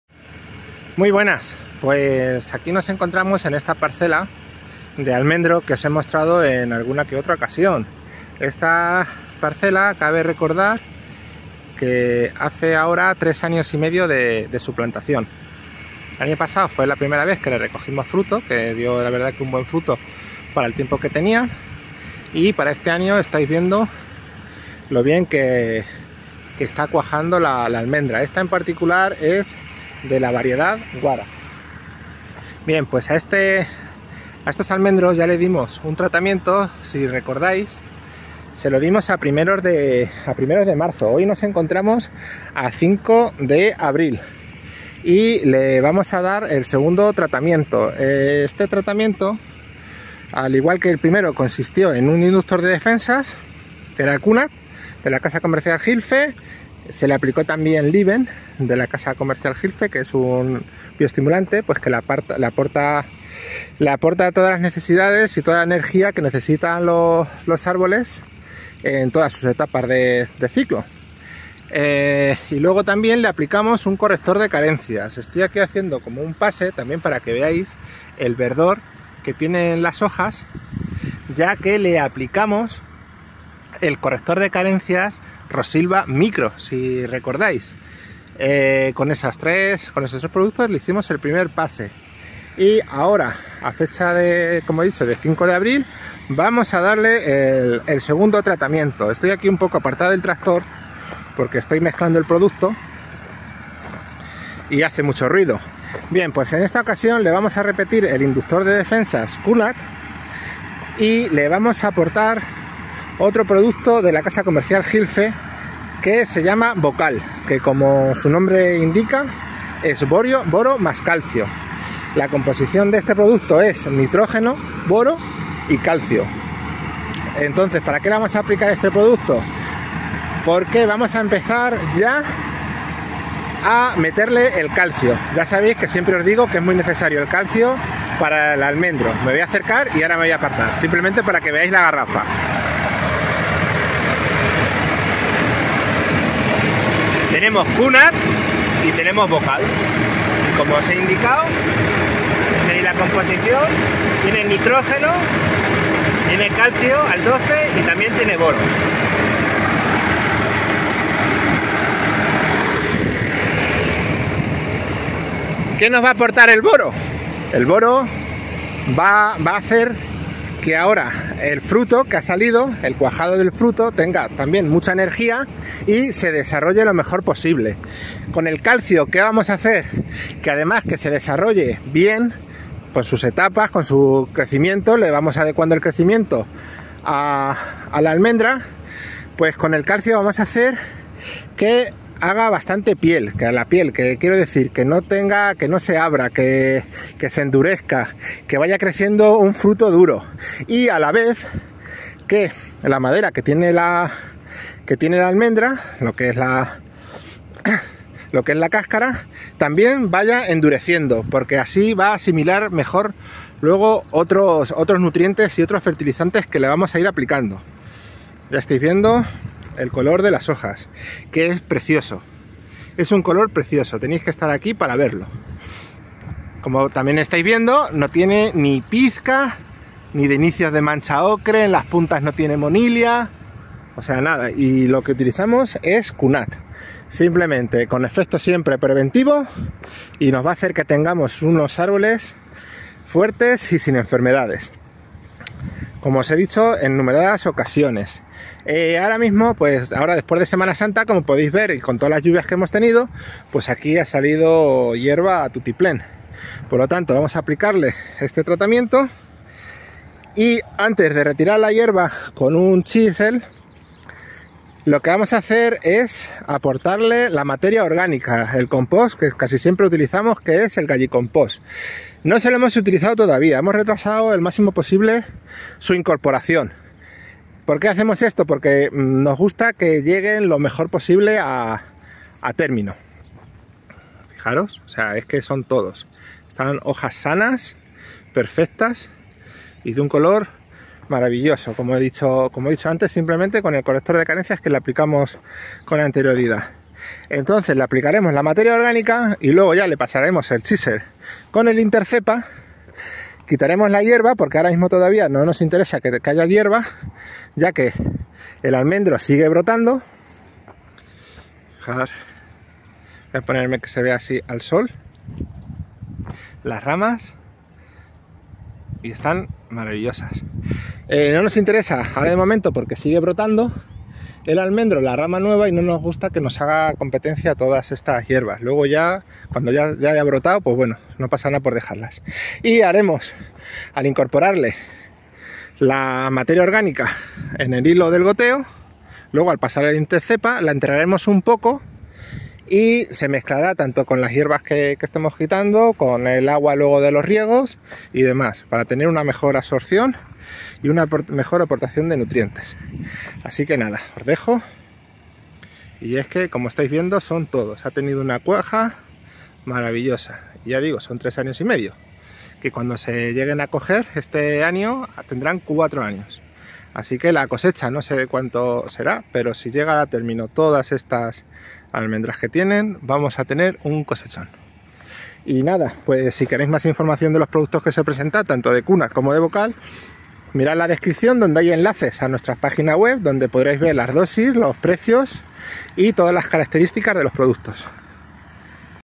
TRATAMIENTO DE ABRIL EN ALMENDRO - Variedad Guara a marco de 7 x 7 m
Tratamiento de Abril en almendros de la variedad Guara plantados a marco de 7 x 7 m, con una dotación de riego de 1200 m3/ha.
Productos utilizados:
CUNAT (Inductor de defensas)
Más información en los teléfonos:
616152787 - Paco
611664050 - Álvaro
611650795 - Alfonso